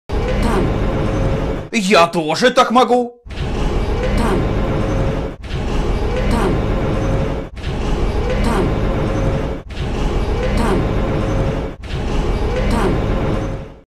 Я тоже так могу
Фрагмент из обзора RedCynic на фильм "Хитмен"